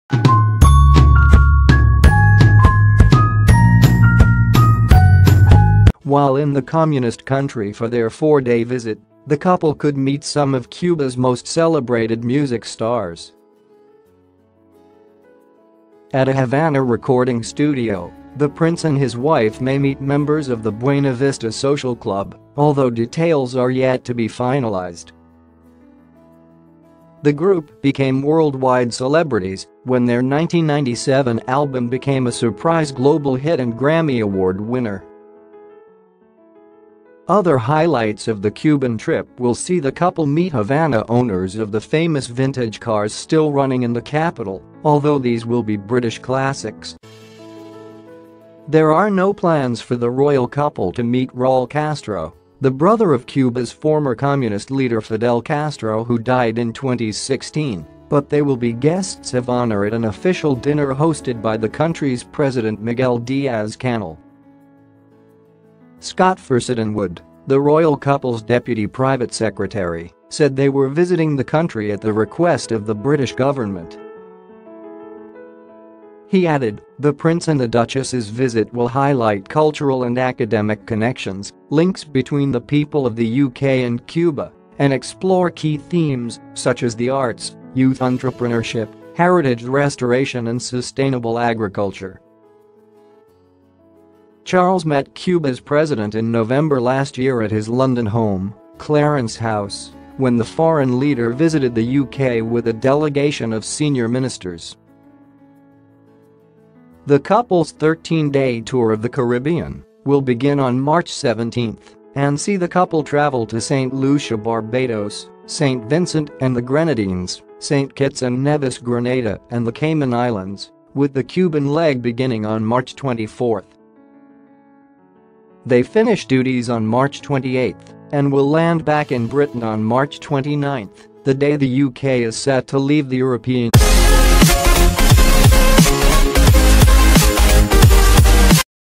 Prince Charles and Camilla to go on Commonwealth charm offensive days-Royal News
While in the communist country for their four day visit, the couple could meet some of Cuba's most celebrated music stars.  At a Havana recording studio, the prince and his wife may meet members of the Buena Vista Social Club, although details are yet to be finalised.  The group became worldwide celebrities when their 1997 album became a surprise global hit and Grammy award winner. Other highlights of the Cuban trip will see the couple meet Havana owners of the famous vintage cars still running in the capital, although these will be British classics. There are no plans for the royal couple to meet Raul Castro, the brother of Cuba's former Communist leader Fidel Castro who died in 2016, but they will be guests of honour at an official dinner hosted by the country's president Miguel Diaz-Canel. Scott Furssedonn-Wood, the royal couple's deputy private secretary, said they were visiting the country at the request of the British Government. He added: "The prince and the duchess' visit will highlight cultural and academic connections, links between the people of the UK and Cuba, and explore key themes, such as the arts, youth entrepreneurship, heritage restoration and sustainable agriculture. "Charles met Cuba's president in November last year at his London home, Clarence House, when the foreign leader visited the UK with a delegation of senior ministers. The couple's 13-day tour of the Caribbean will begin on March 17 and see the couple travel to St Lucia, Barbados, St Vincent and the Grenadines, St Kitts and Nevis, Grenada and the Cayman Islands, with the Cuban leg beginning on March 24. They finish duties on March 28, and will land back in Britain on March 29, the day the UK is set to leave the European Union.